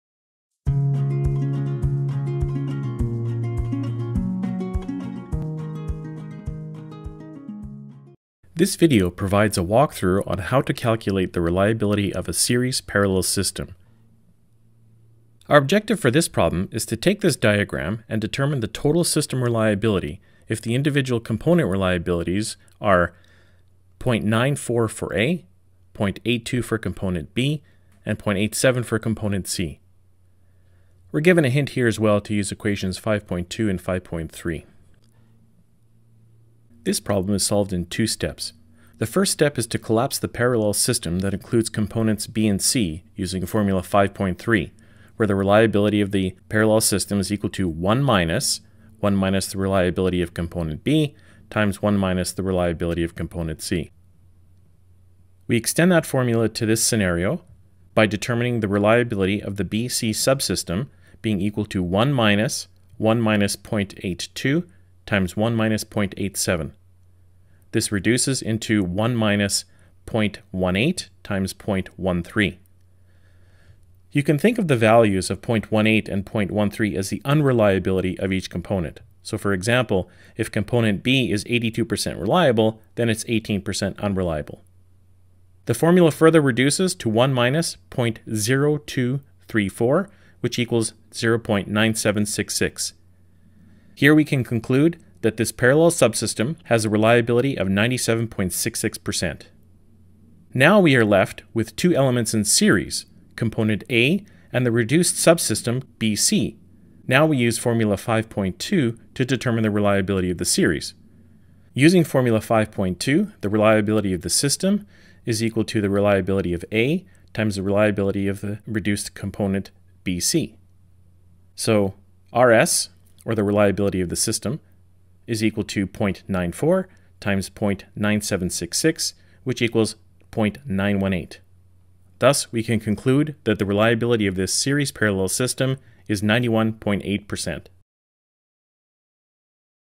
Series-Parallel System Reliability
This video will illustrate how to calculate the reliability of a series—parallel system.